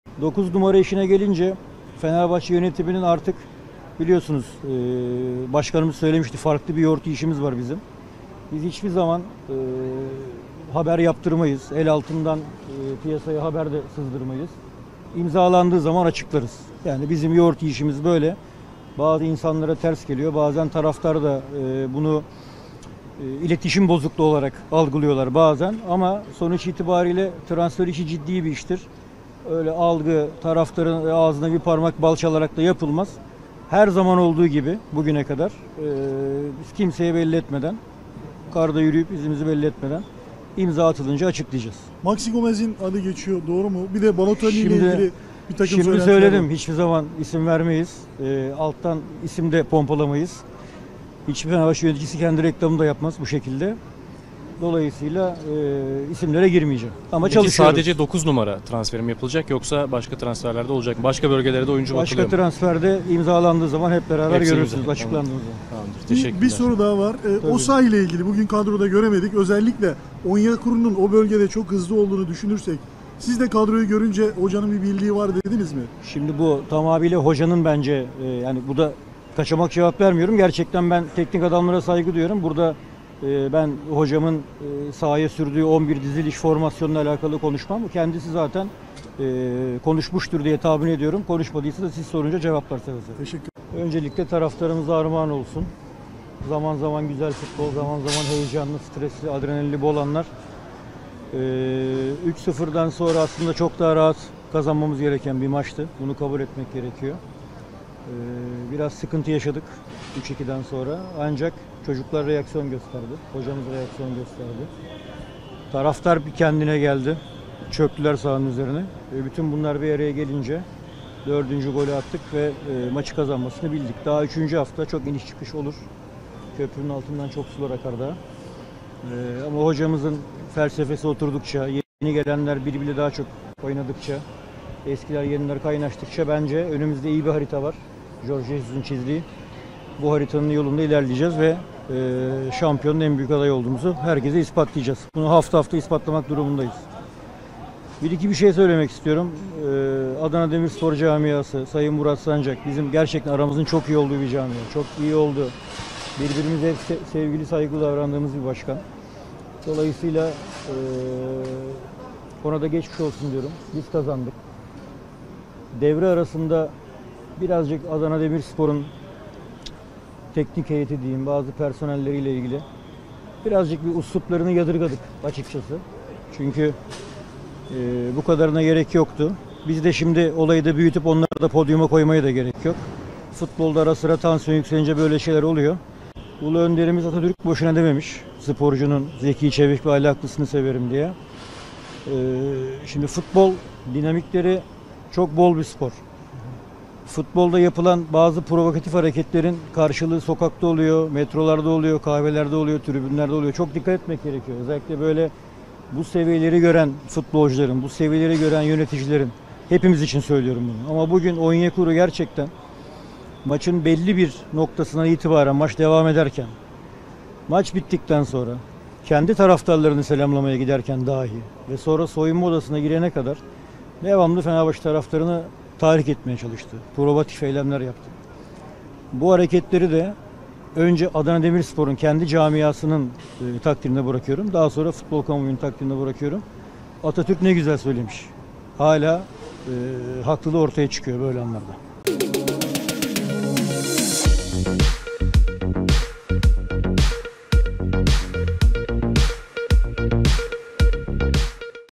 Selahattin Baki, Maxi Gomez sorusuna ne demişti? "Bizim yoğurt yiyişimiz farklı"
Fenebahçe'de Selehattin Baki'nin geçtiğimiz hafta forvet transferi için yaptığı açıklama.